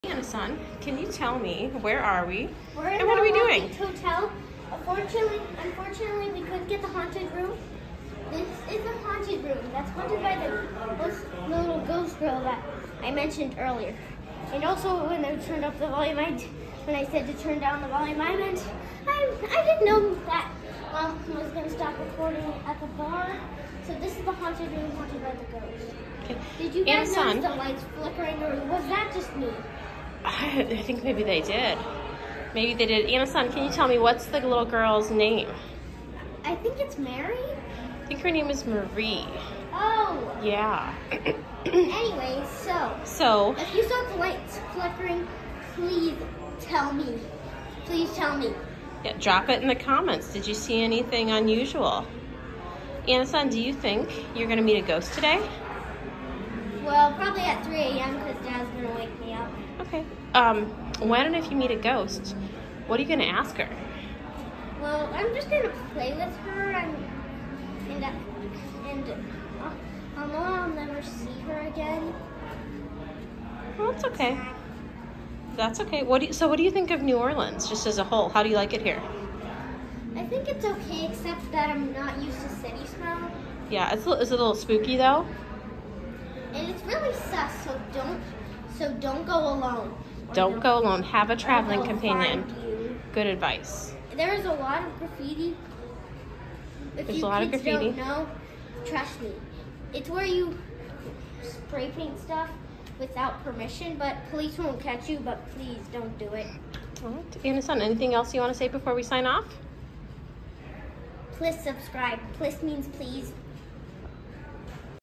Ghost hunting at the Lafitte Hotel
Anna Sun is hoping to meet a ghost that haunts the Lafitte Hotel.  She is apparently a little girl that died of yellow fever known to like visiting children who stay in the French Quarter hotel.